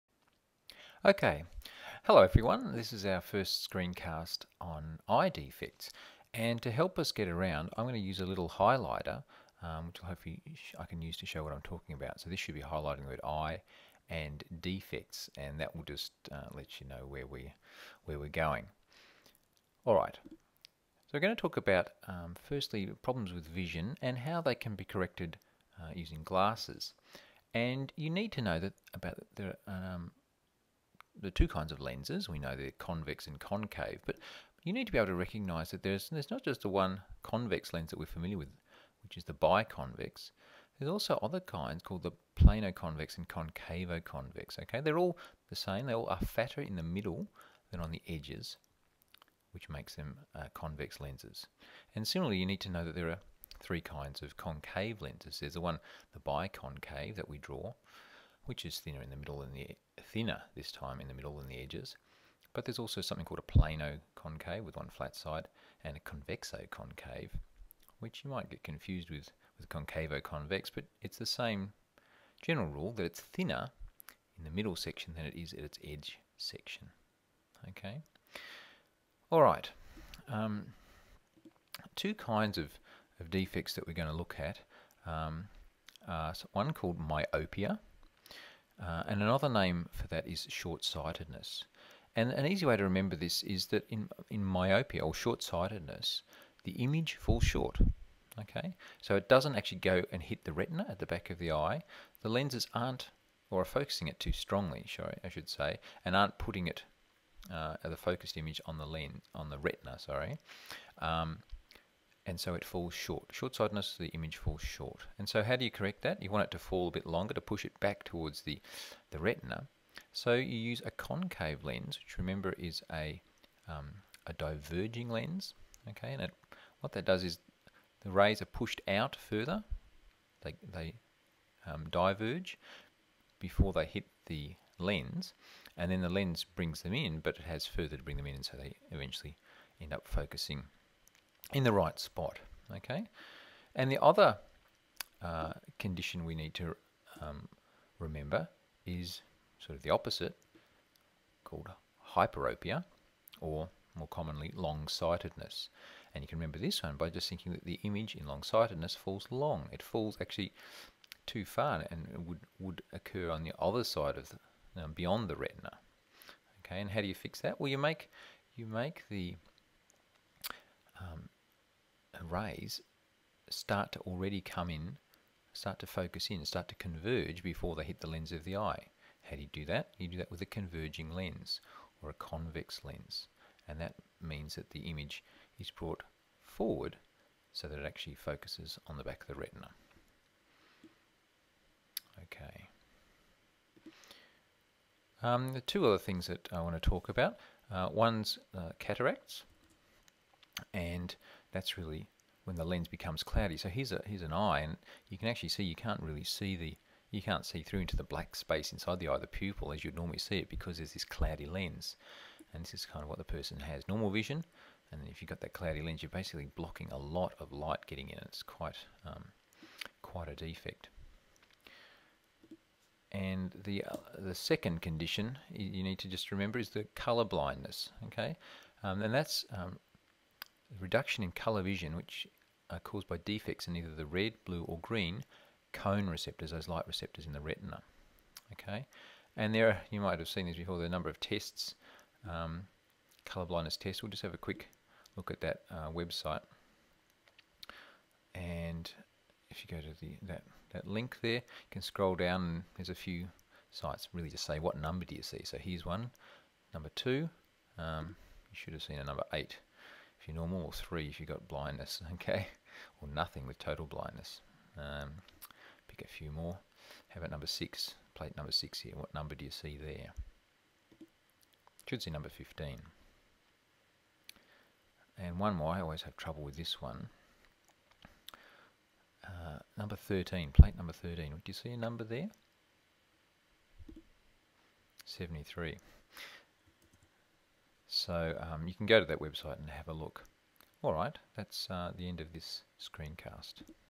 Eye Defects
Short sightedness, Long sightedness, Cataracts and Colour Blindness